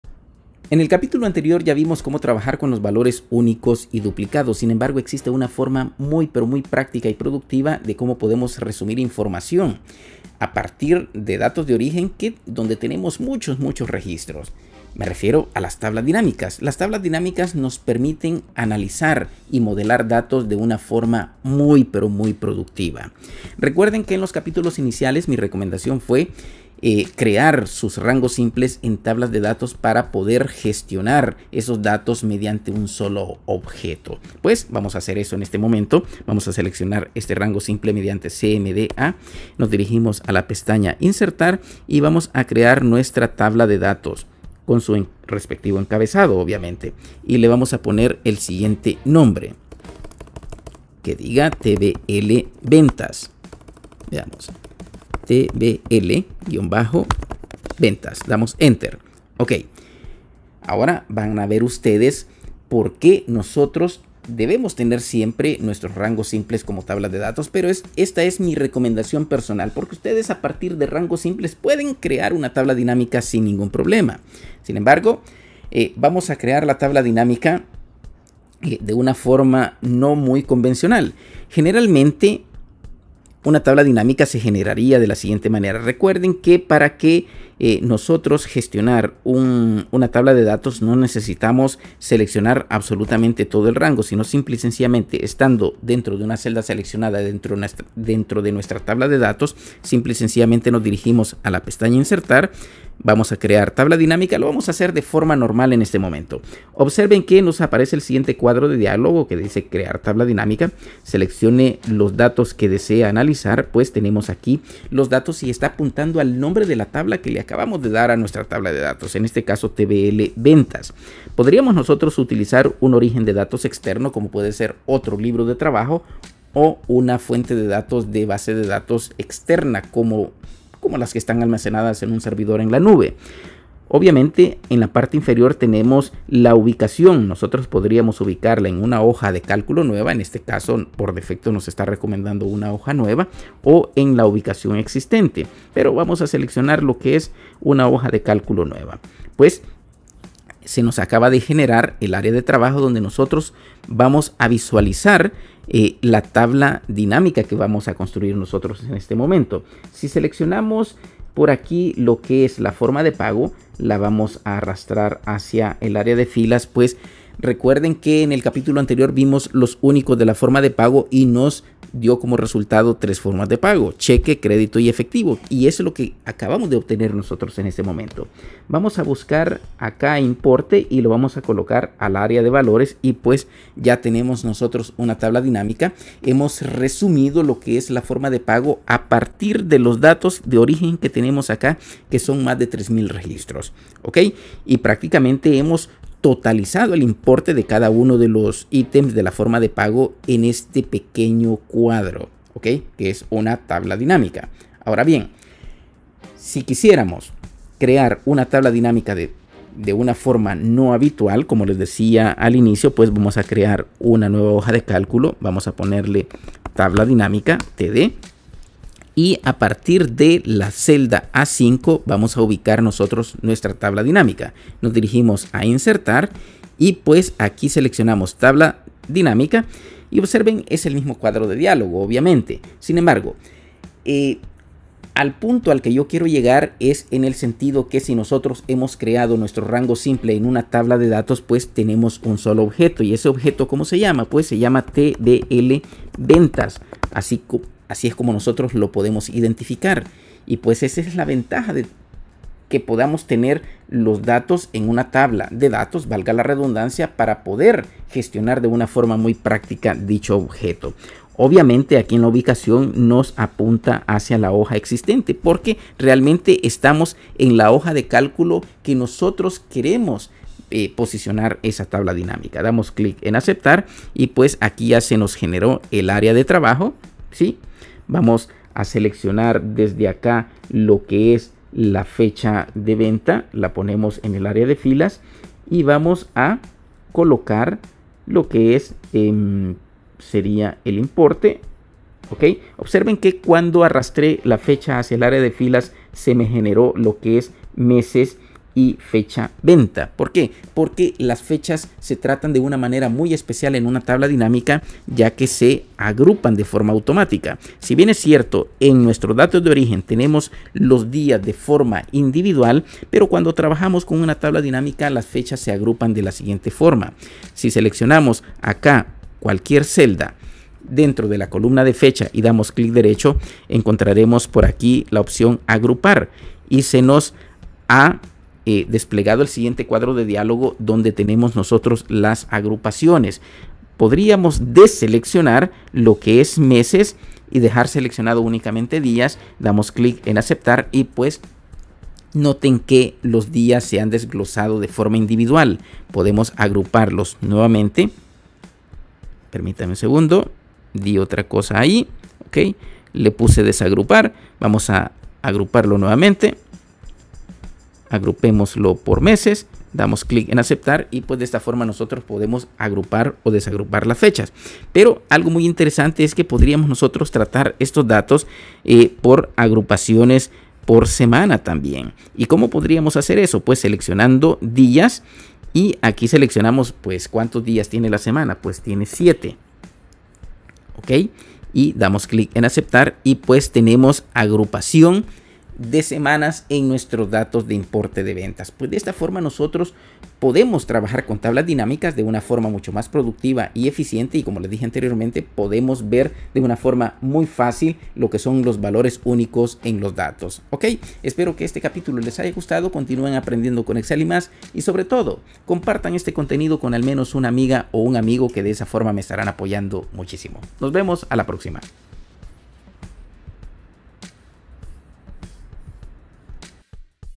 CAP-11 | Creando Tablas Dinámicas | Curso de Excel para macOSS
Las tablas dinámicas son parte importante en el análisis de datos en Excel, lo cual te permitirá ser más productivo en tu día a día laboral.